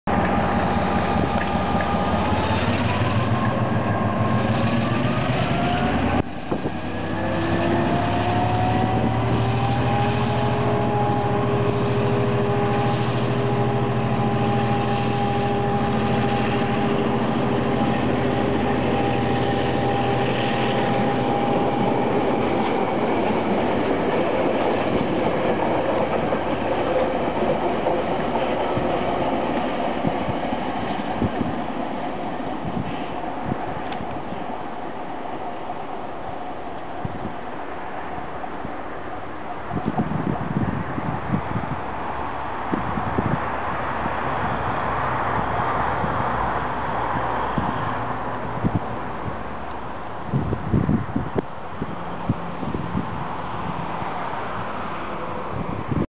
Train Departs from New Platform at Swanson
During the double tracking construction project in West Auckland in 2008, we see DC 4916 departing from the new second platform at Swanson, while work continues in the foreground on replacing the old original track, and extending the older platform.  The project was completed, and both tracks in use, from June 2008.